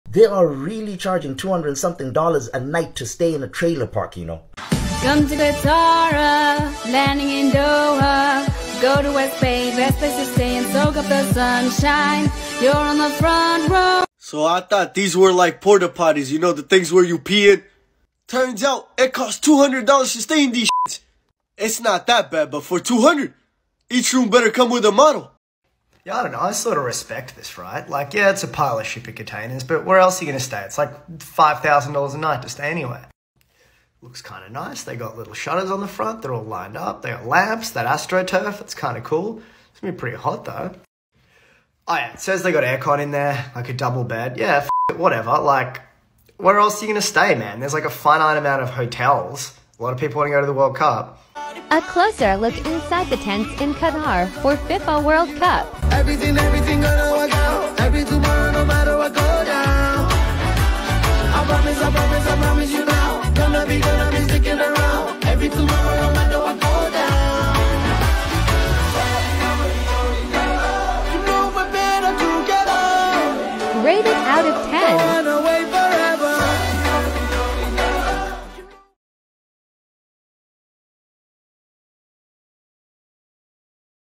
FIFA World Cup: What it's like to stay in Qatar's Al Emadi fan village
Under-resourced and overpriced, fans react to the conditions in Qatar.